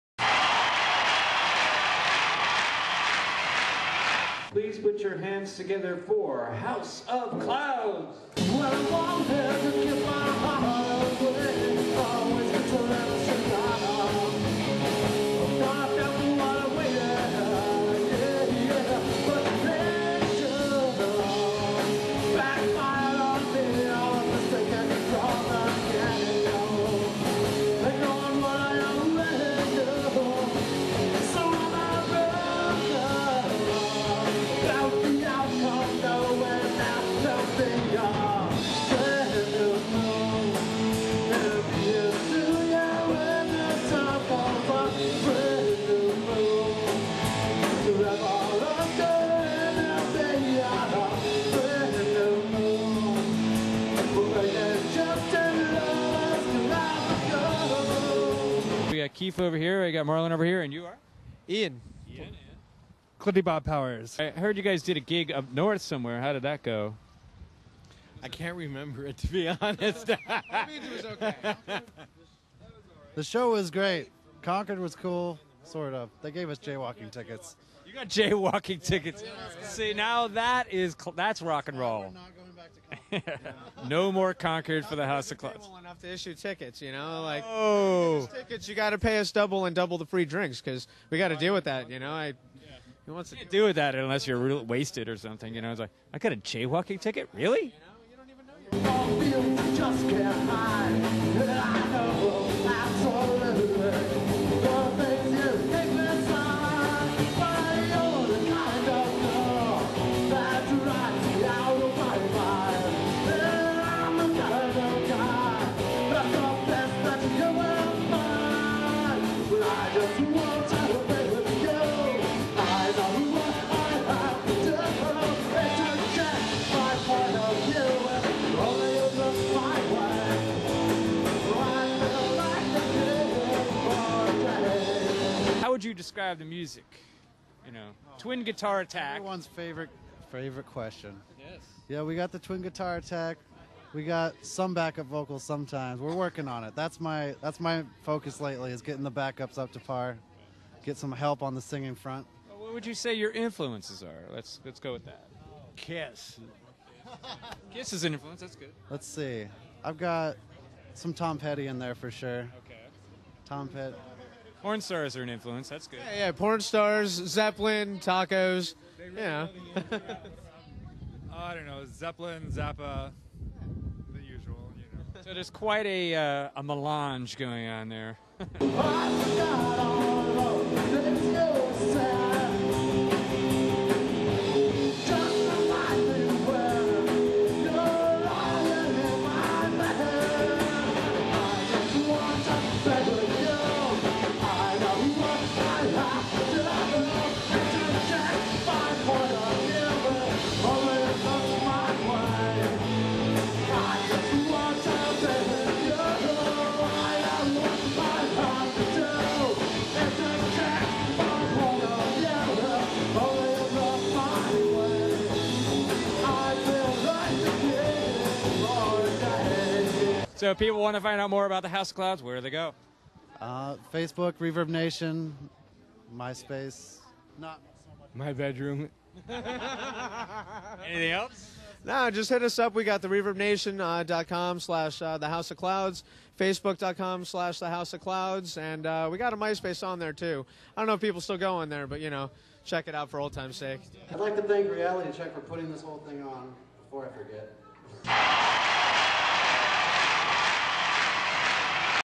House of Clouds at Haight Street Fair Battle of the Bands (2011)
Outtake from episode #507: at the 2011 Haight Street Fair Battle of The Bands. House of Clouds featuring members of the Nirv & Bella Mortis and roommates of the Reality Check TV guys!! These guys write catchy tunes that are chapters in the continuing saga of Rock n' Roll!